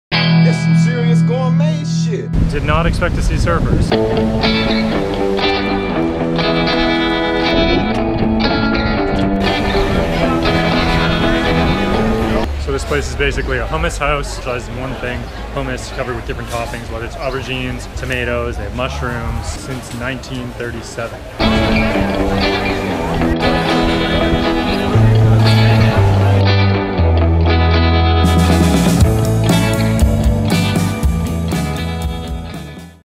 AMAZING HUMMUS IN TEL AVIV - Mantry Vlog #99
Mantry Vlog #99 - The Mantry team heads to Shlomo & Doron, a  hummus spot in Tel Aviv,  there are surfers too.